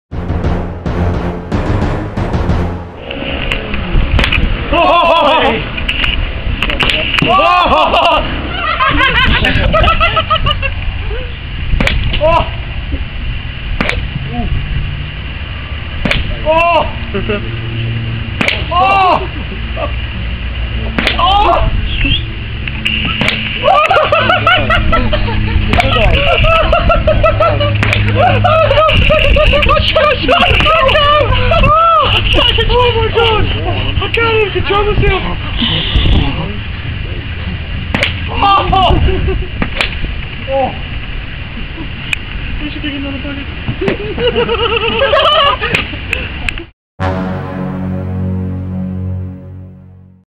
Airbourne Drop Whack
After a few fine local Queenstown ales, us men thought a cheeky peruse of the local driving range could be in order. Once spirits were high, the festivities begun. Enjoy.